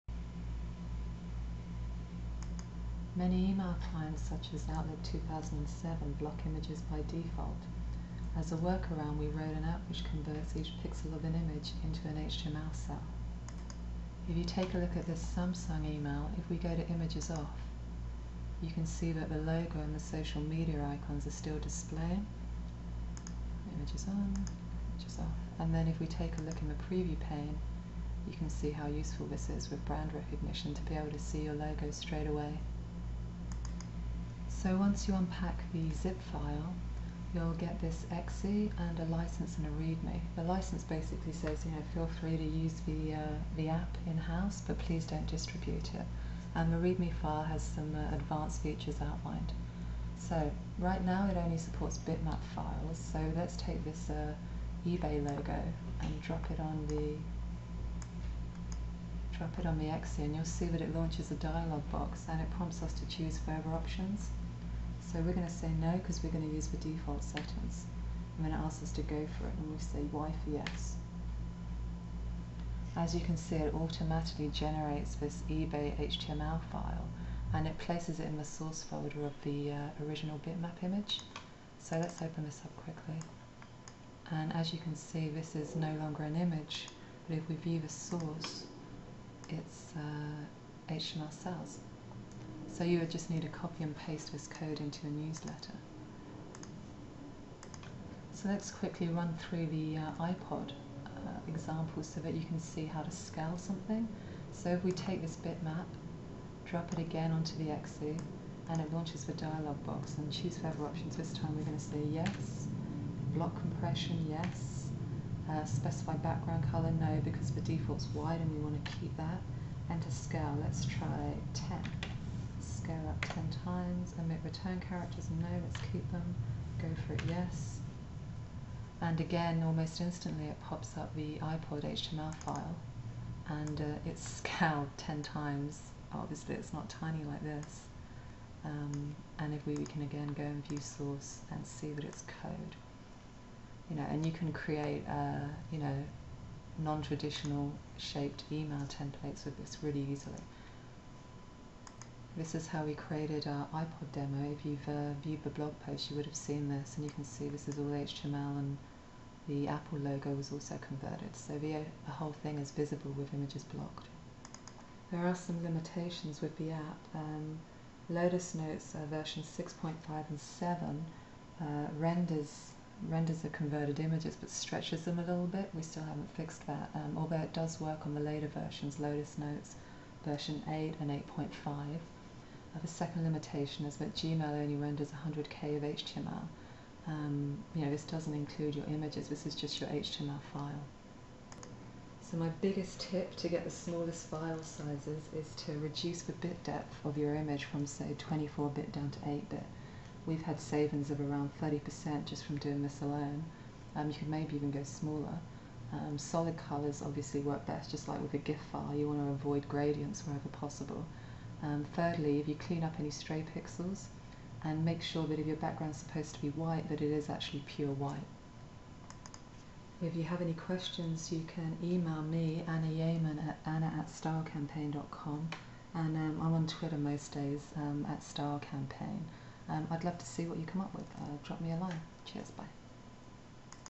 Image to HTML Converter for Email - STYLECampaign
Many email clients such as Outlook 2007 and Gmail block images by default.

As a solution, we wrote an app that converts each pixel of an image into a corresponding HTML cell. It works in all email clients , except for Lotus Notes 6.5 & 7. It also works on the iPhone.

To forward yourself some demos and find out more about converting images to HTML go to: